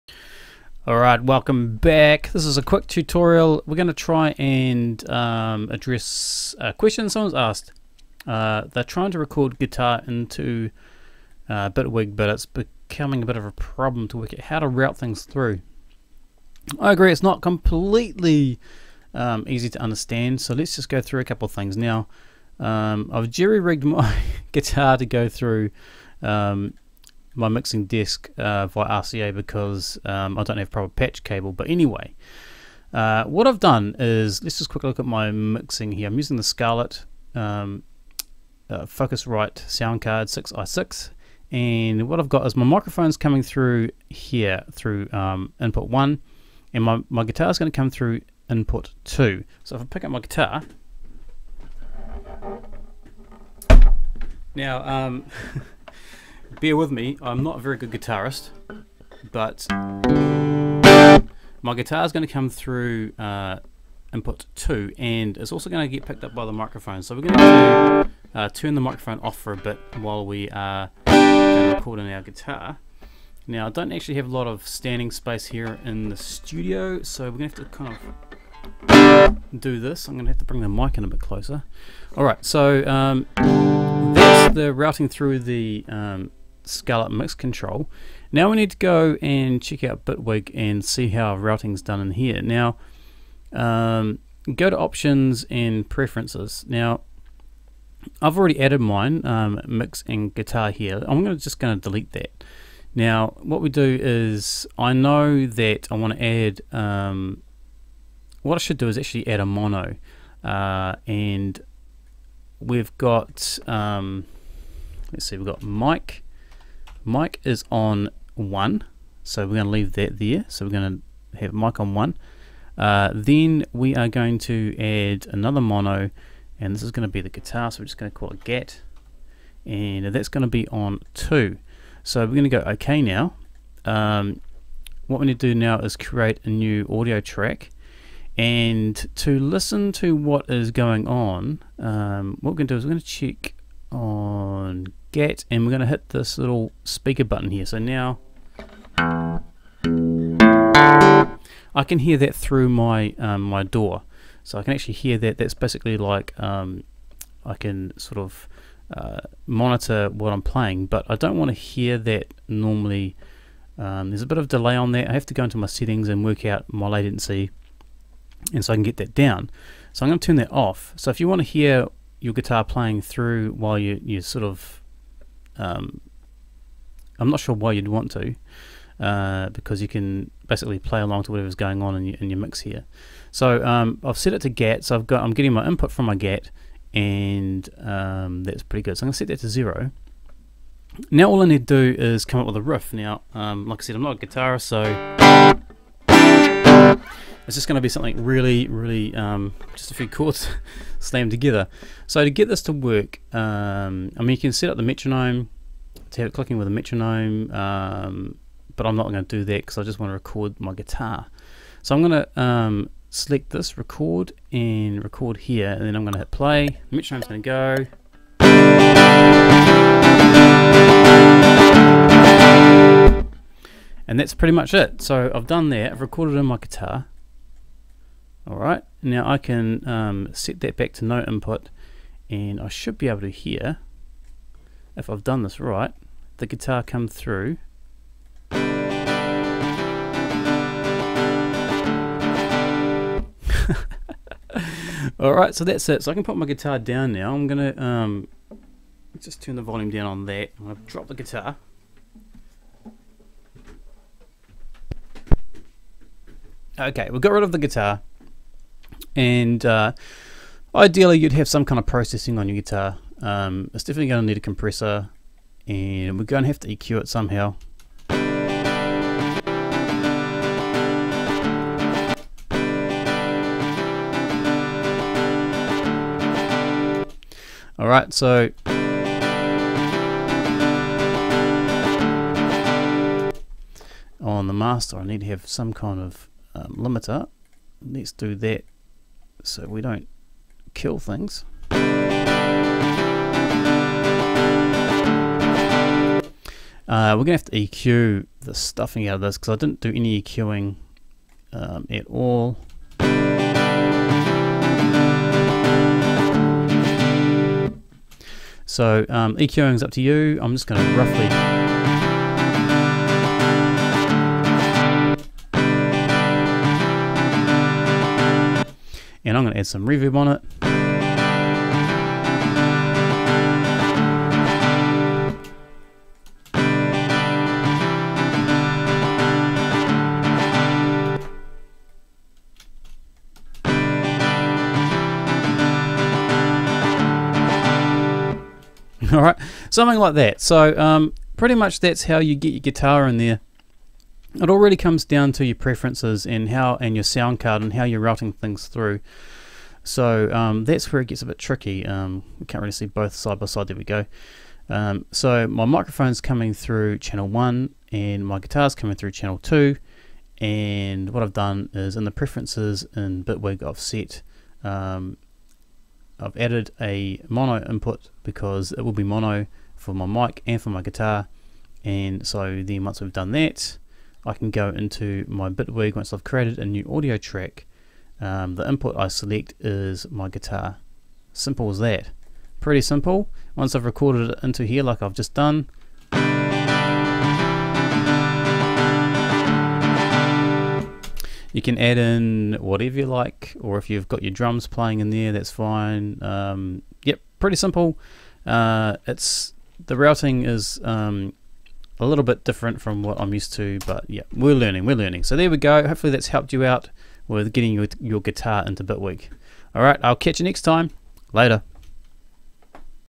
Bitwig Tutorial - Recording Guitar into Bitwig
I need to record my Guitar into Bitwig but first I need to setup the Audio preferences in Bitwig to allow me to select the correct input for recording. The Volume may be a bit loud.. Sorry.